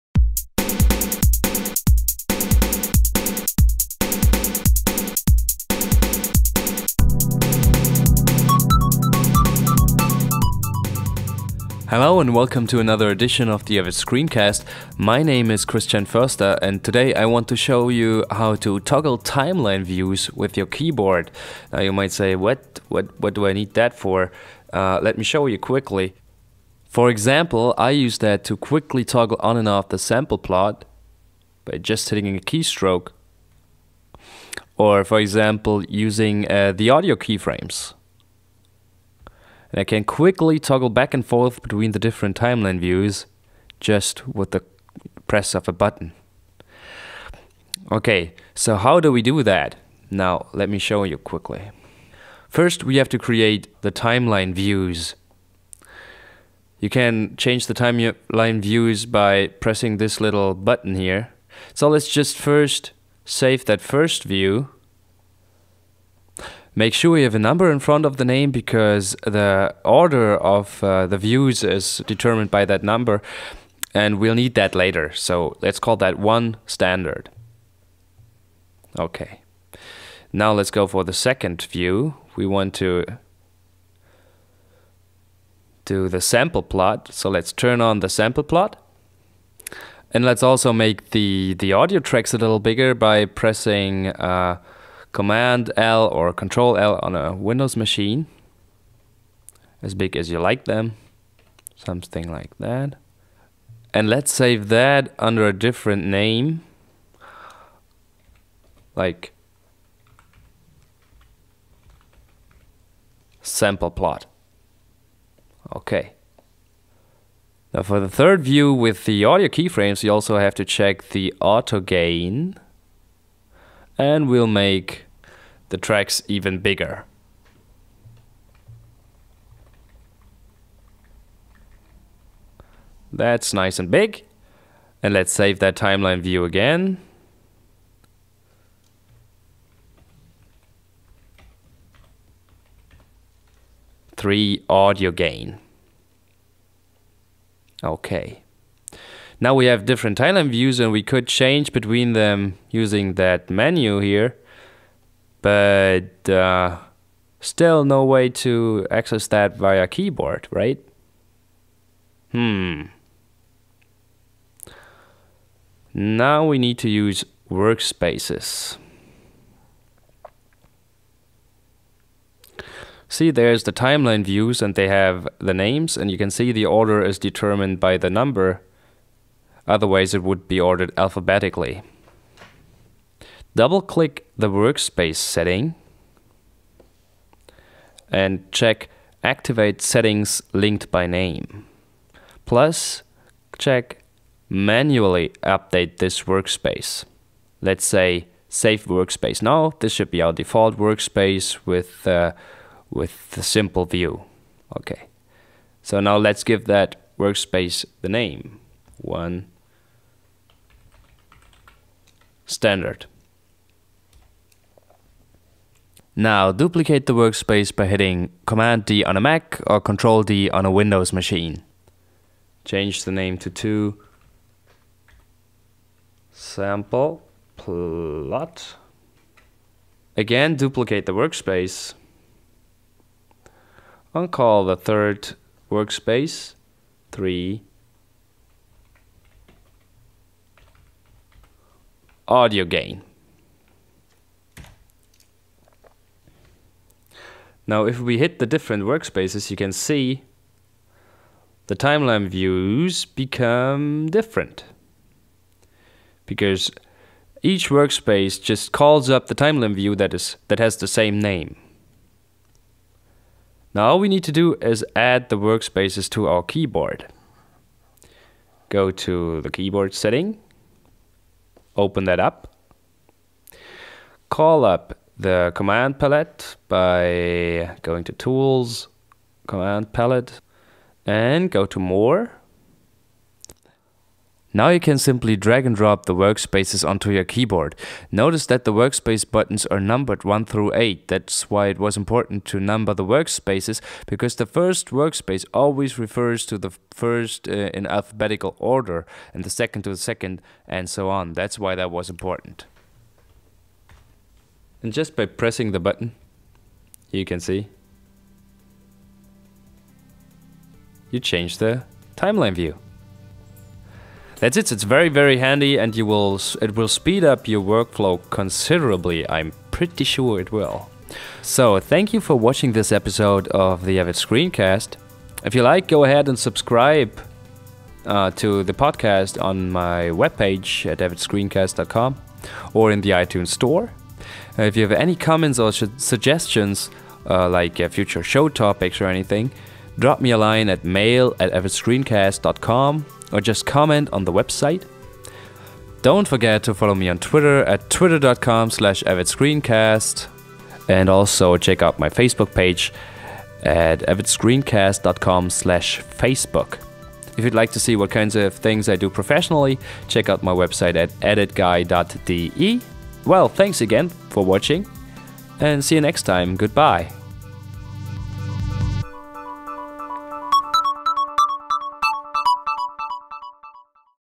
Avid Screencast #02: Toggling Timeline Views With Your Keyboard
In this episode, we'll increase your productivity by being able to quickly switch between different timeline views. For example, just press a button on your keyboard and automatically enlarge the audio tracks while turning on the audio waveforms. It's quick and easy!